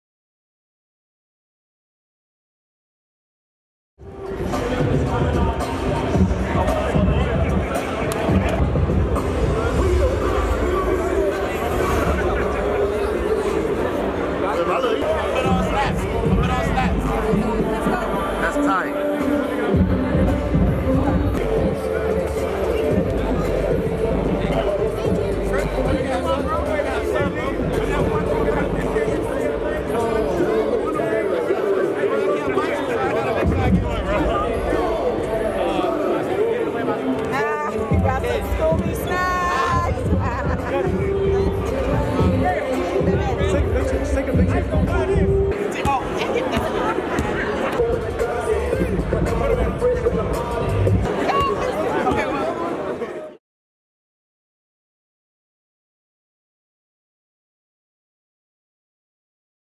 Jaylen Hands Gives Jordan Bell Scooby Snacks | Dreamers
Compton Magic's stand out star PG is on to the next step at UCLA, but he had one more time to be a HS player and showed out in front of his family and friends. Someone gave him some Scooby Snacks and Jordan Bell was quick to grab one haha. We love all the players in this video and we want to thank Compton Magic for everything they do.